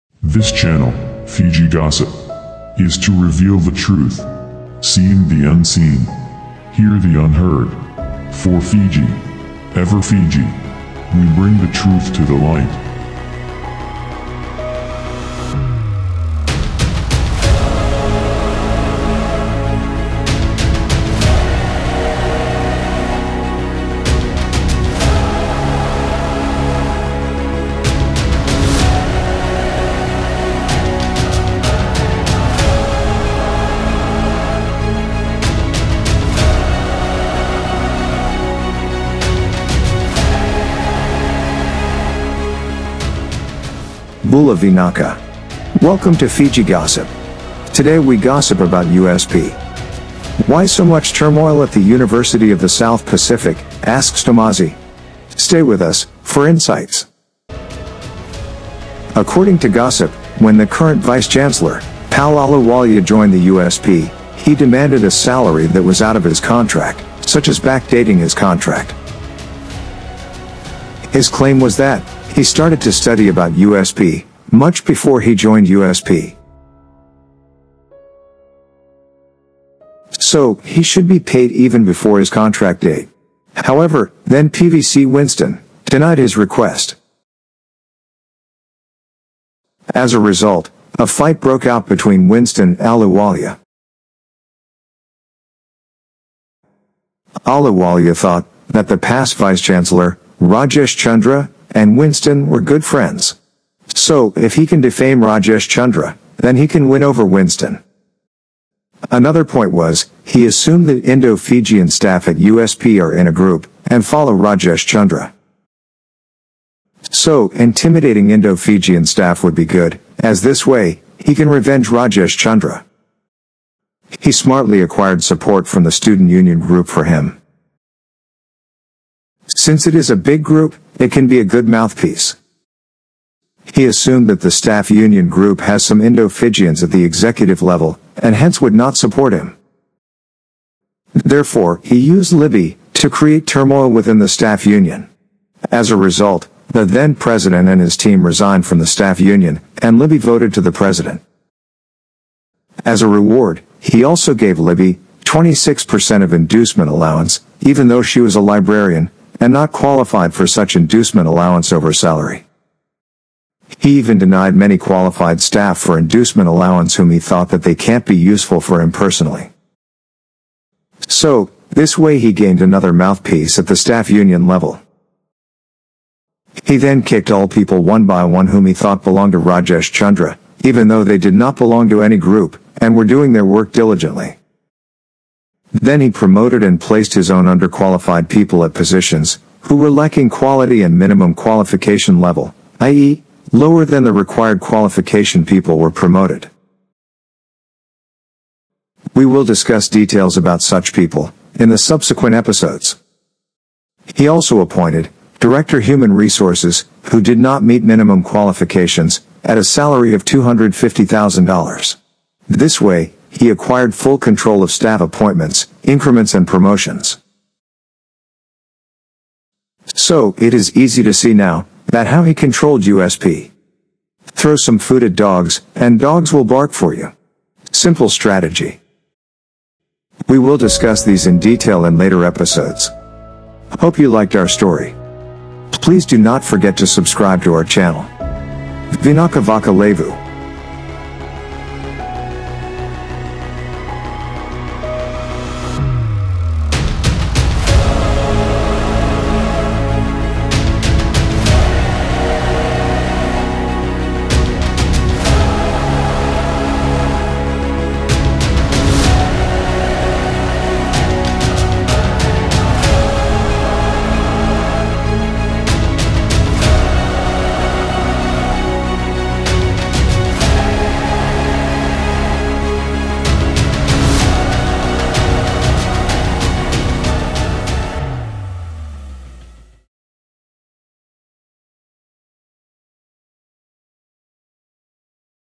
Fiji Gossip Episode 1: Why turmoil at University of the South Pacific (USP)? Behind Story
Episode 1
Bula Vinaka! Welcome to Fiji Gossip!
We're talking about the University of the South Pacific today!
Why is there so much upheaval at the USP?
Aren't we getting skewed news or only the news that we want to hear? What is the truth behind all of these classifications?
Are political parties or key members of political parties participating, and if so, why? Is there a secret motive here? Let us seek out and attempt to bring the truth to the public's attention.

The University of the South Pacific (USP) is a regional university in the south Pacific. It has been in operation for almost 50 years and is one of Fiji's best institutes. Its council is made up of representatives from 12 small island states. Fiji is one of the most generous donors to USP. Over the previous few decades, it has seen many Vice-Chancellors. USP has a large number of alumni in Fiji. As a result, it is extremely important to the people of Fiji.